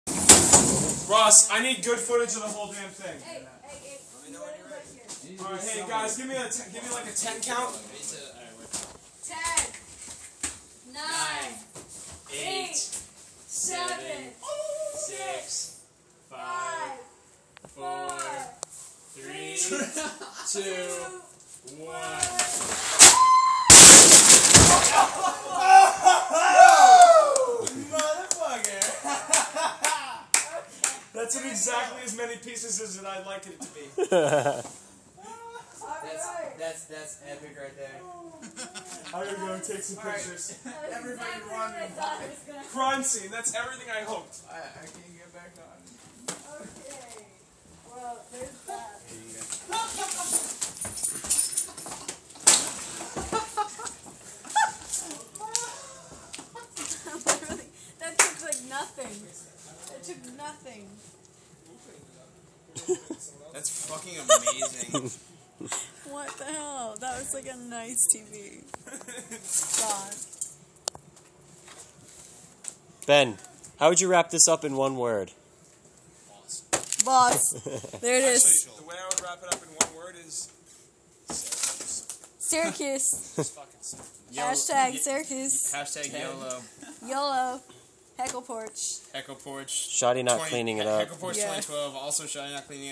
Big Screen TV Kicked Off Second Floor Balcony
Moving out from our college apartment, we didn't feel like carrying the TV down the stairs so we tossed it over the balcony.